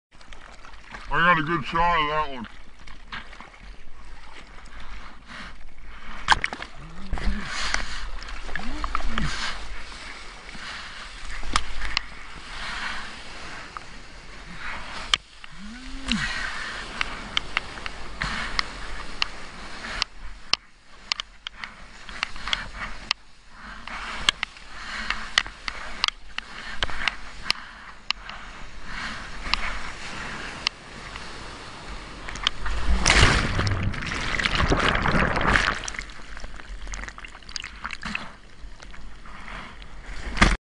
Trace: Surfing - Connor Stevenson at Del Mar
Trace auto edited Connor Stevenson's surfing session at Del Mar on Aug 05, 2015.

Waves: 34
Min of Surfing: 337.5
Distance: 1,513 yd
Calories Burned: 656
Max Speed: 18.7 mph